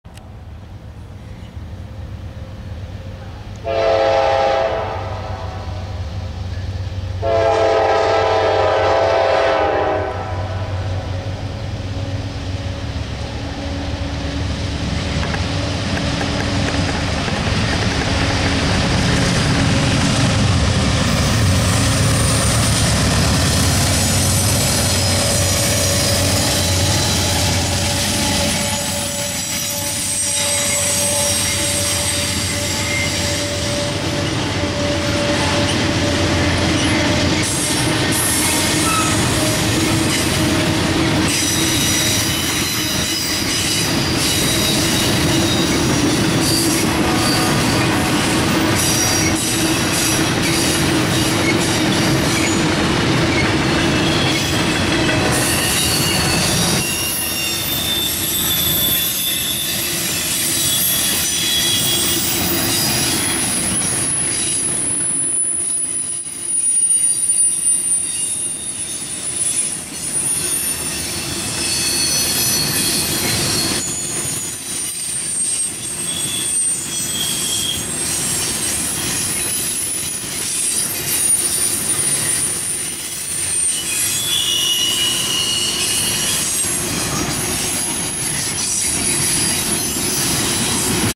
Norfolk Southern trailer train through Alliance, Ohio
Video taken July 10th, 2015. Nothing special on this trailer train, NS 9707 and 9457 both C44-9Ws was the power.